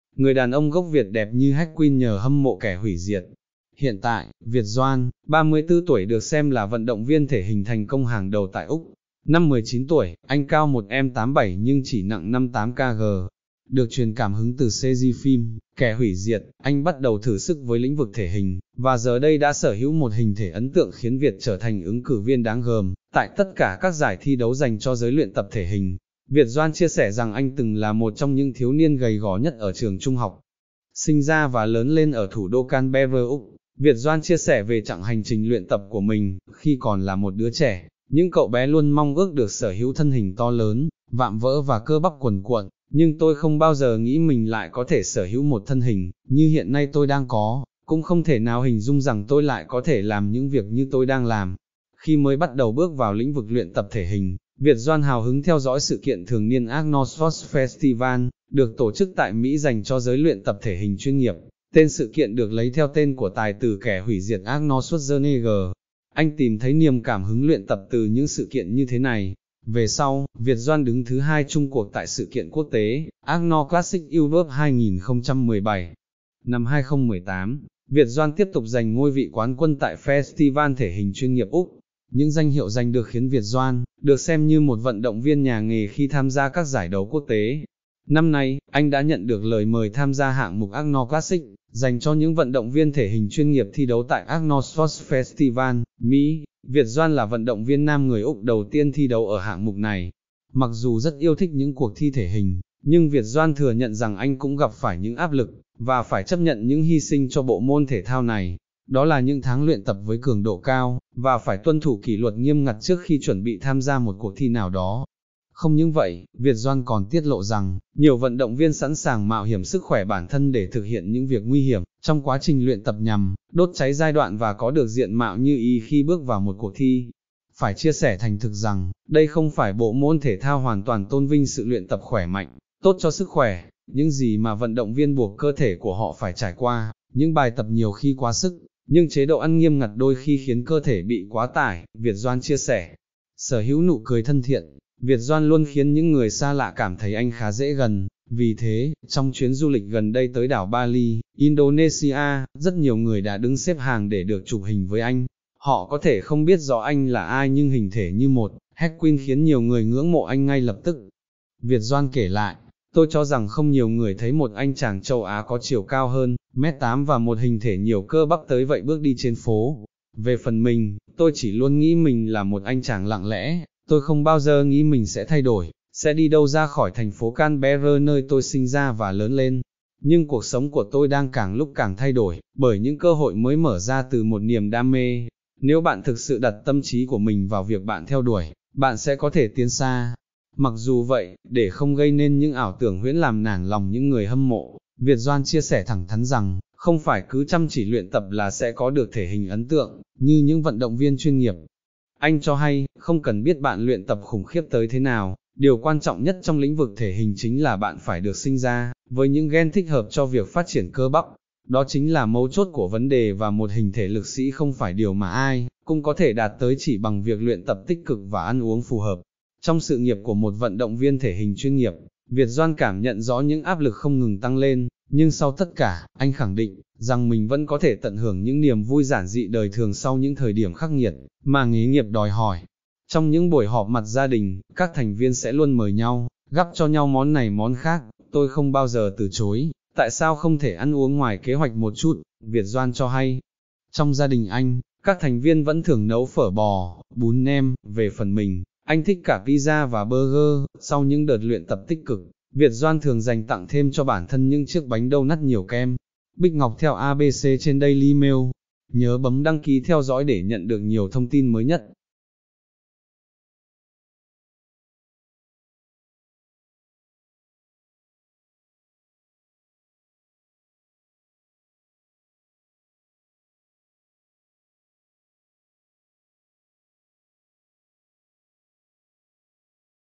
Người đàn ông gốc Việt đẹp như “Héc-quyn” nhờ hâm mộ “Kẻ hủy diệt”
(Dân trí) - Một người đàn ông gốc Việt từng hâm mộ tài tử “Kẻ hủy diệt” Arnold Schwarzenegger đã quyết định thử sức với việc luyện tập thể hình. Giờ đây, anh được xem là vận động viên thể hình hàng đầu tại Úc. Từ một niềm hâm mộ, Viet Doan đã làm thay đổi cuộc đời mình.